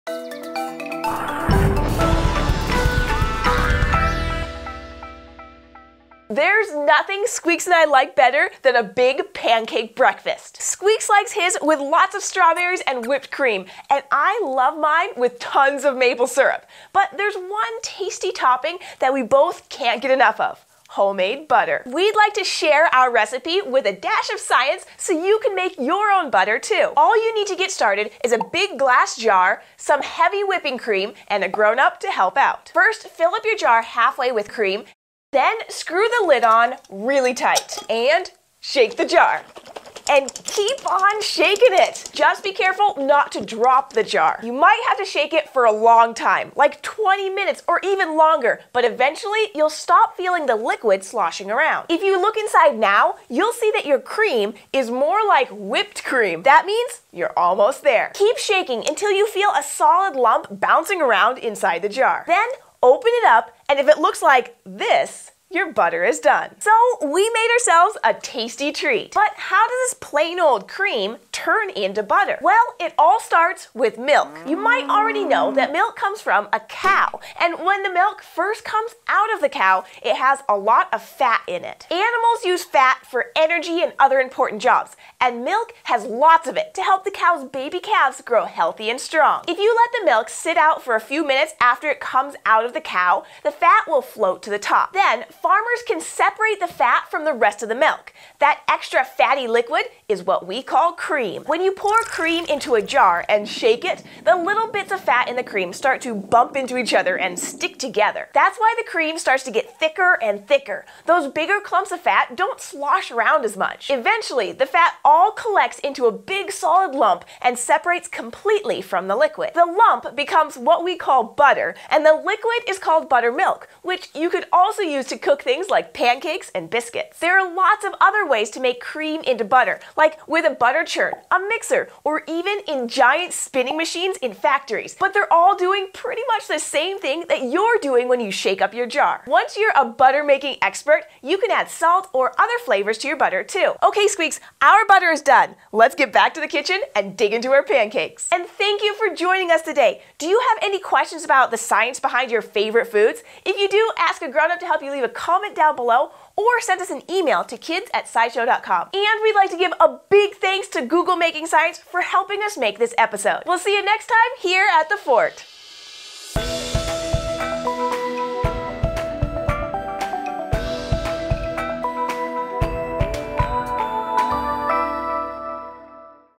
Let's Make Butter!
Jessi and Squeaks are having pancakes, but first they have to make a key ingredient: butter!